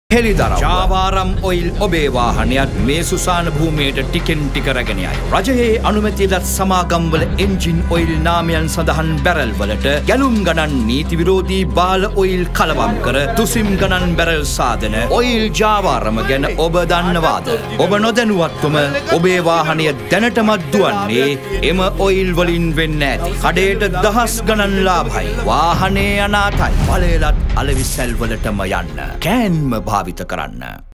Caltex - "හෙලිදරව්ව" TV Commercial
Caltex - "හෙලිදරව්ව" is a campaign conducted by Chevron Lubricant Lanka PLC to make aware the public regarding the adulterated engine oil trade in Sri Lanka and educate them how to get rid of it.